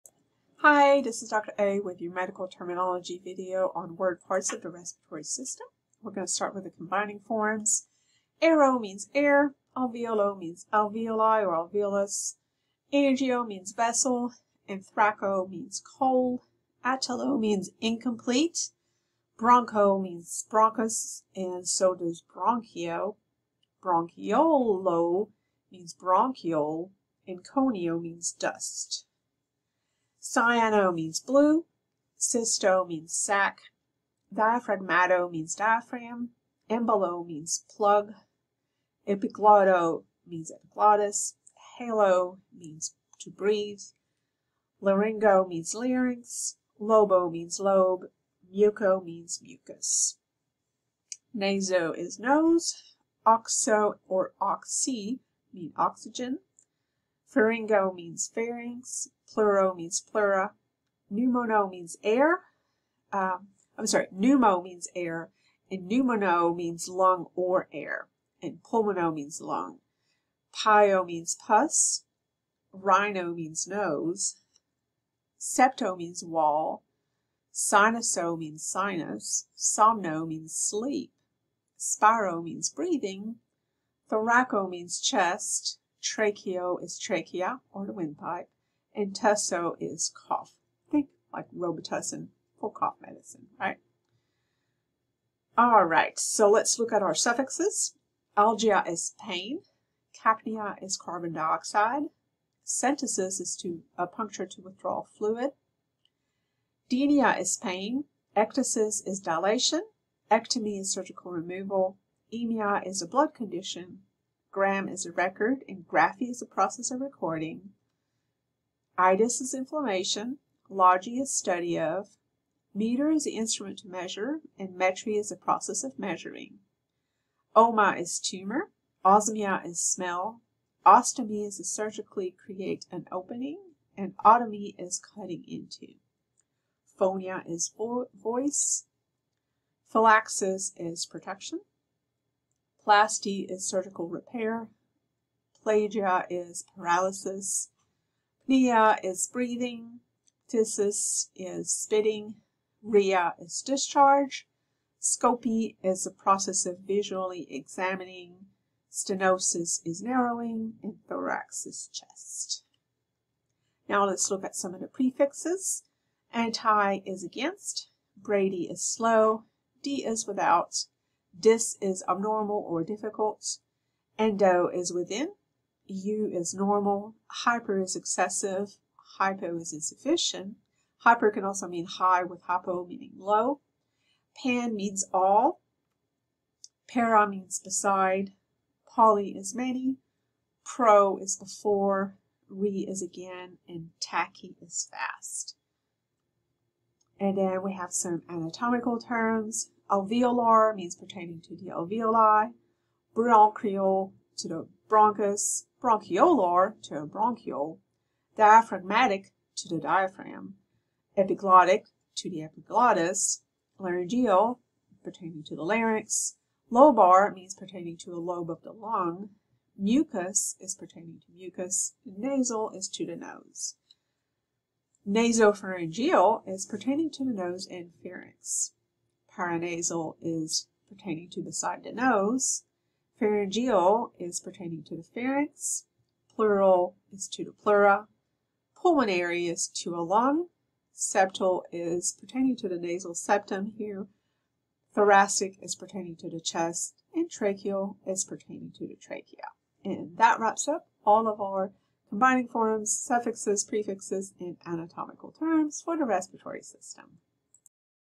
Medical Terminology Word Parts of the Respiratory System
The combining forms, suffixes, prefixes, and anatomical terms that pertain to the respiratory system.

Based on Medical Terminology: A Living Language by Fremgen and Frucht. Published by Pearson.